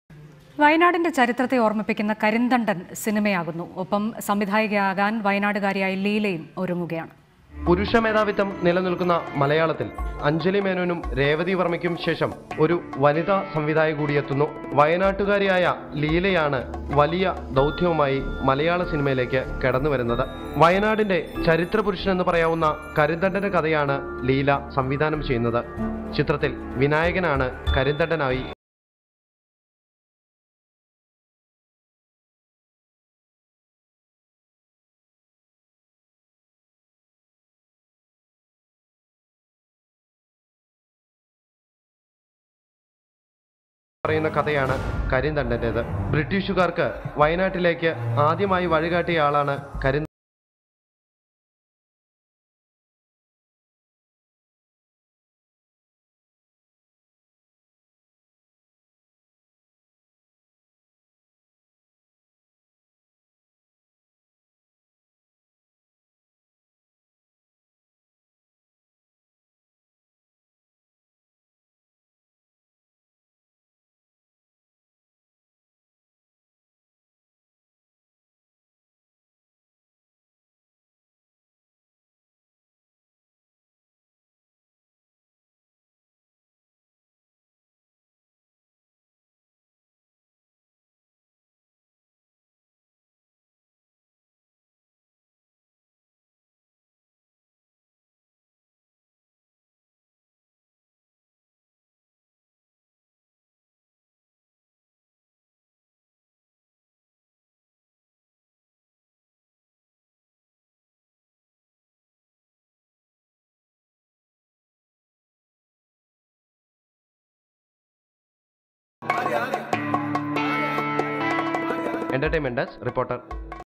കരിന്തണ്ടന്റെ കഥ സിനിയാകുന്നു, സംവിധായിക ലീല_Malayalam Latest News_Reporter Live
Team Reporter